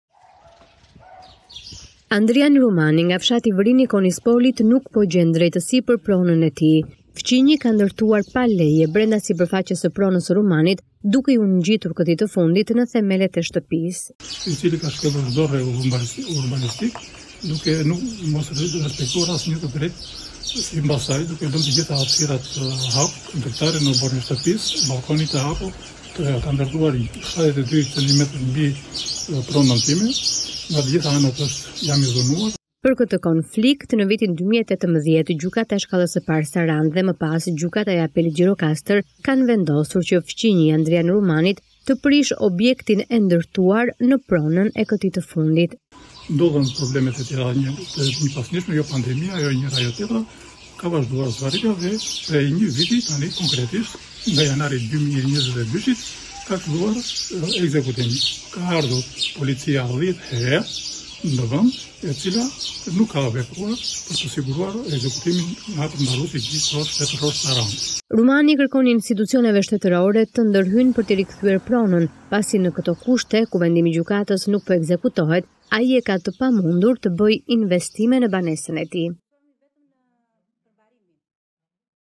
Në konflikt për pronën/ Fqinji i ndërton në tokën e tij, banori nga Konispoli kërkon drejtësi
Një banor nga Konispoli kërkon drejtësi për pronën. Fqinji i ka zënë tokën duke ndërtuar një objekt pa leje por edhe pse gjykatat kanë vendosur në favor të tij ndërtesa nuk është shembur ende. Andrijan Rumani nga fshati Vrinë i Konispolit nuk po gjen drejtësi për pronën e tij.

Fqinji ndërtuar një subjekt 72 cm brenda sipërfaqes së pronës së Rumanit duke ju ngjitur këtij të fundit në themelet e shtëpisë. "Ka shkelur çdo rregull urbanistik duke mos resopektuar hapsirat, duke lënë dritare në oborrin e shtëpisë, ballkonin hapur. Nga të gjitha anët jam i dhunuar," tha ai.

Për këtë konflikt ne vitin 2018 Gjykata e Shkallës së parë Sarandë dhe më pas Gjykata e Apelit Gjirokastër kanë vendosur që fqinji i Andrijan Rumanit të prish subjektin e ndërtuar në pronës e këtij të fundit.

"Duken problemet e vazhdueshme, jo pandemia, njëra tjetra. Ka vazhduar zvarritja dhe prej nëj viti nga janar 2022 ka filluar ekzekutimi. Ka ardhur policia 10 herë në vend dhe nuk ka vepruar për të siguruar ekzekutimin," tha ai.

Rumani i kërkon institucioneve shtetërore të ndërhyjnë për ti rikthyer pronën pasi në këto kushte ku vendimi i gjykatës nuk po ekzekutohet ai e ka të pamundur të bëj investime në banesën e tij.